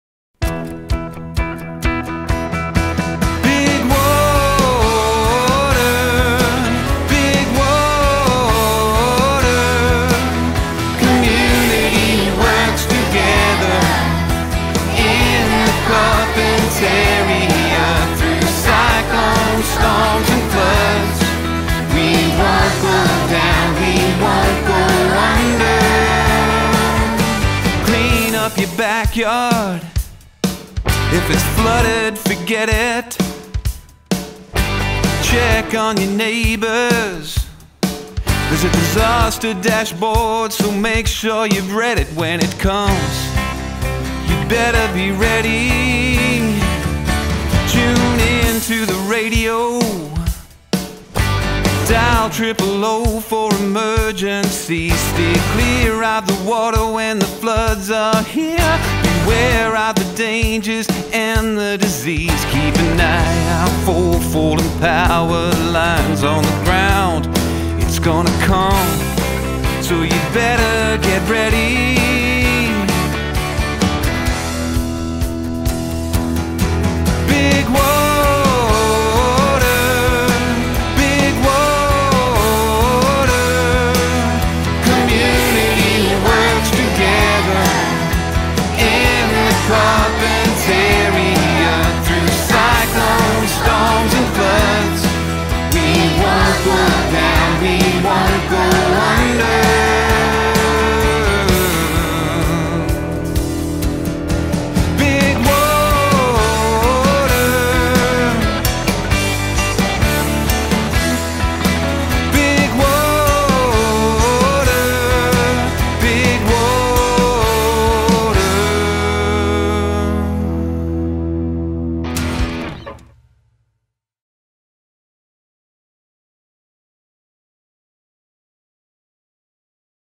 Big Water (Carpentaria Shire Council)
The 'Big Water' Production has been an initiative by Carpentaria Shire Council to raise awareness in the local community for disasters such as floods and cyclones. It was great to involve the locals from each town as well as students from the 3 Schools in the region, Normanton State School, Gulf Christian College and Karumba State School.